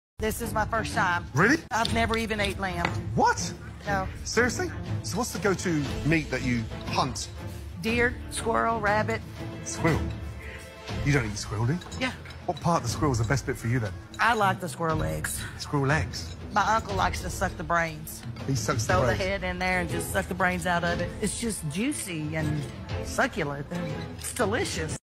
🐿️👨‍🍳 Gordon Ramsay's Shock: Contestant's Unusual Taste for Squirrel Leaves Him Disturbed! 🌟😲
🐿️🤢 Watch Ramsay's expression of disbelief as he learns that a contestant enjoys eating squirrel, a culinary preference that challenges his culinary norms. Can the seasoned chef keep his composure, or will this unexpected revelation leave him utterly baffled? 🤔🔥